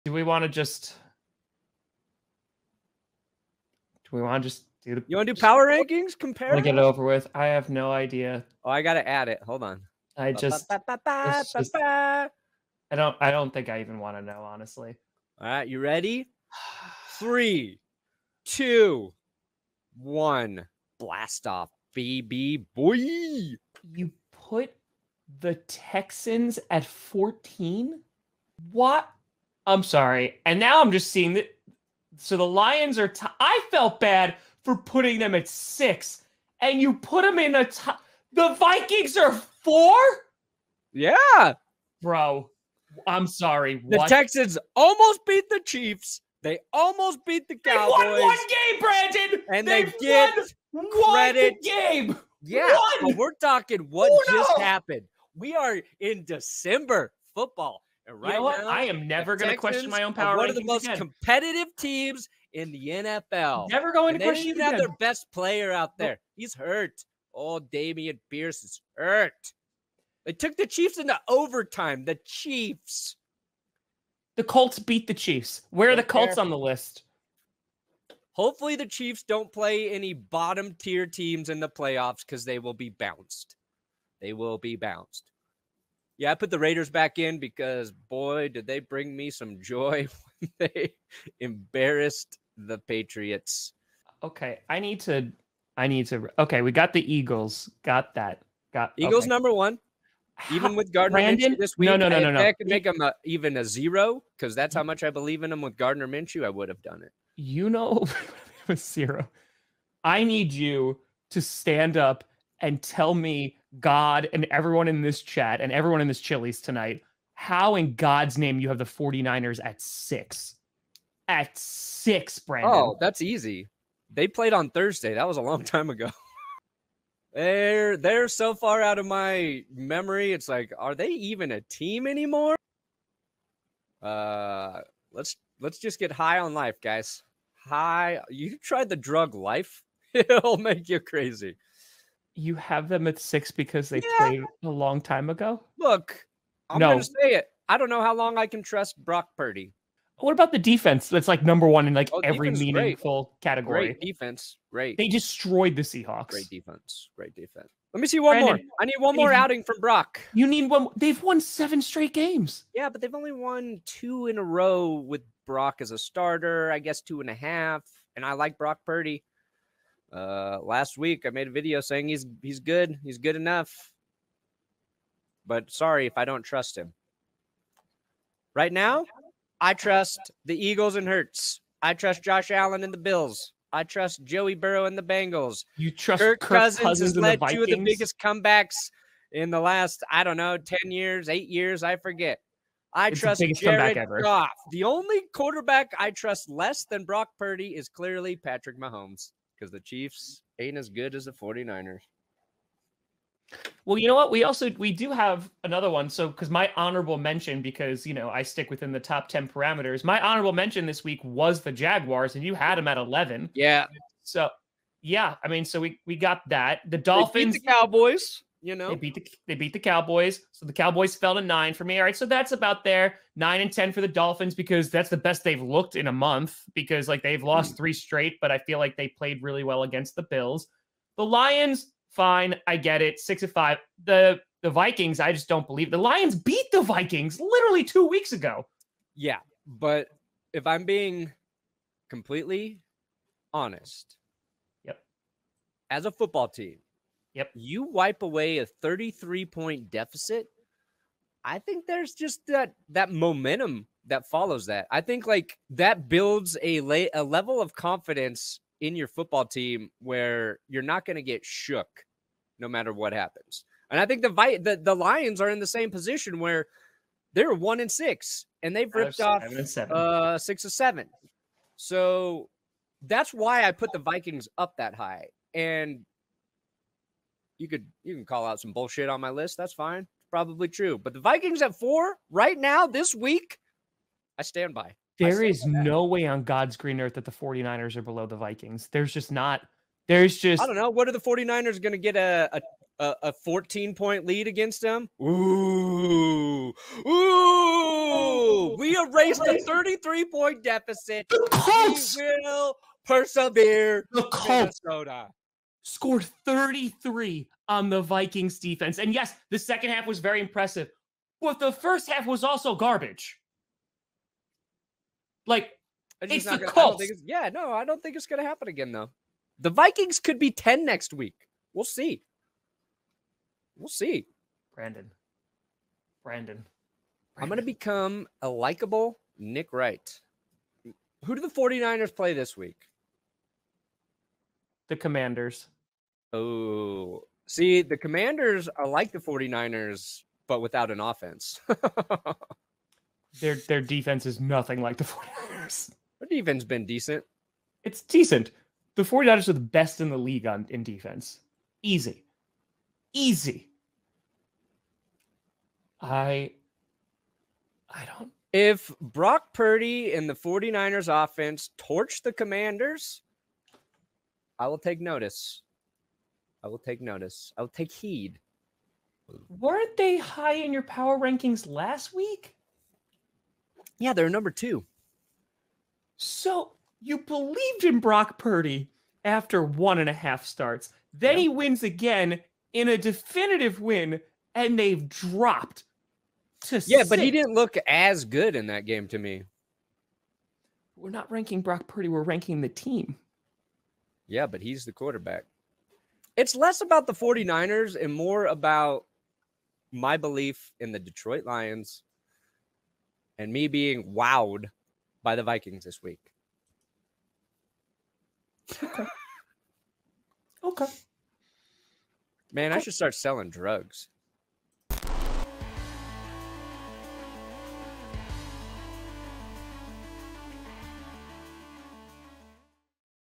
Perna's Power Rankings Are Driving Tom Insane
GPS includes Tom Grossi of Packast and Brandon Perna of ThatsGoodSports